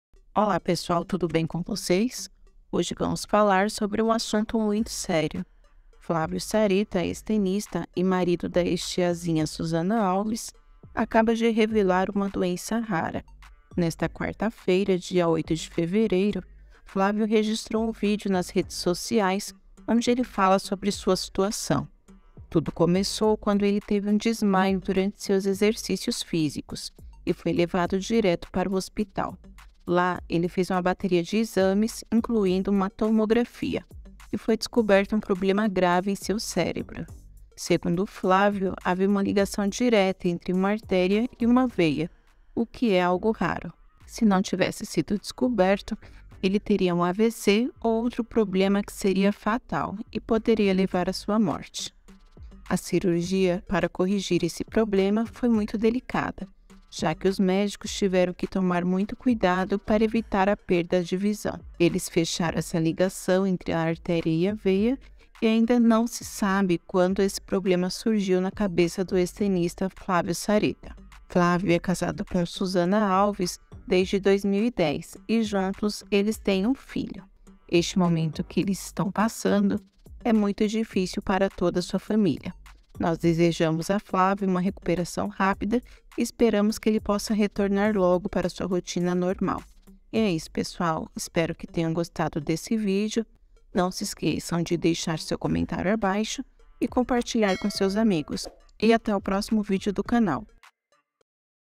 URGENTE! FLÁVIO SARETTA, MARIDO DE SUZANA ALVES, DESCOBRE DOENÇA RARA - FOFOCA DOS FAMOSOS
Marido de Suzana Alves, a Tiazinha, Flávio Saretta descobre doença rara.

"Poderia ser Fatal"

 Hoje vamos falar sobre um assunto muito sério.

O marido da ex-Tiazinha Suzana Alves, Flávio Saretta, acaba de revelar uma doença rara.

Tudo começou quando ele teve um desmaio durante seus exercícios físicos e foi levado direto para o hospital.

Lá, ele fez uma bateria de exames, incluindo uma tomografia, e foi descoberto um problema grave em seu cérebro.

Segundo Flávio, havia uma ligação direta entre uma artéria e uma veia, o que é algo raro.

Se não tivesse sido descoberto, ele teria um AVC ou outro problema que seria fatal e poderia levar à sua morte.

A cirurgia para corrigir esse problema foi muito delicada, já que os médicos tiveram que tomar muito cuidado para evitar a perda de visão.

Eles fecharam essa ligação entre a artéria e a veia, e ainda não se sabe quando esse problema surgiu na cabeça do ex - tenista Flávio Saretta.

Flávio é casado com Suzana desde 2010, e juntos eles têm um filho.

Este é um momento muito difícil para toda a sua família.

Nós desejamos a Flávio uma recuperação rápida e esperamos que ele possa retornar logo para sua rotina normal.

E é isso pessoal, espero que tenham gostado deste vídeo.

 Até o próximo vídeo do CANAL!

flavio saretta